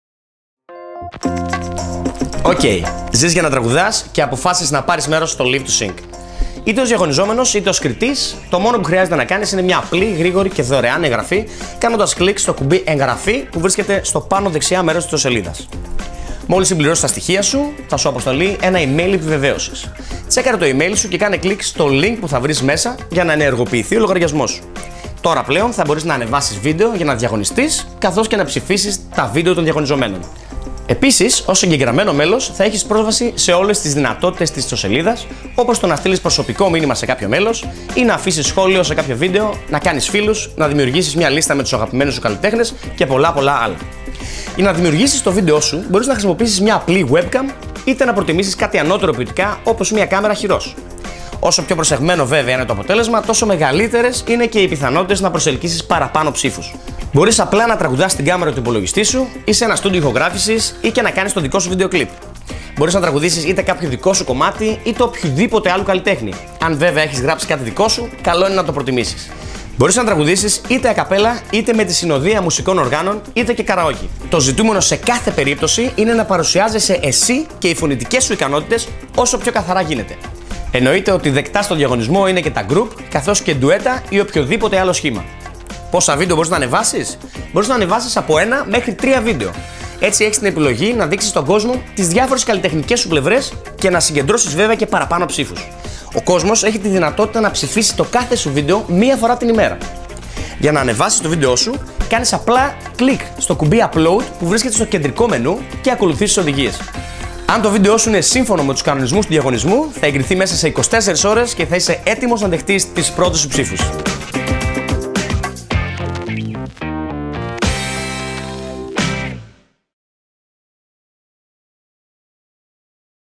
Οδηγίες για το Live2Sing
Οι συμμετοχές ξεκίνησαν, ανέβασε το βίντεο με την ερμηνεία σου και κέρδισε συμβόλαιο με δισκογραφική, δωρεάν βίντεο κλιπ και €10.000!
Περισσότερα βίντεο στο κανάλι του Live2Sing.

Μπες στο για να δηλώσεις συμμετοχή ή να ψηφίσεις τους διαγωνιζόμενους!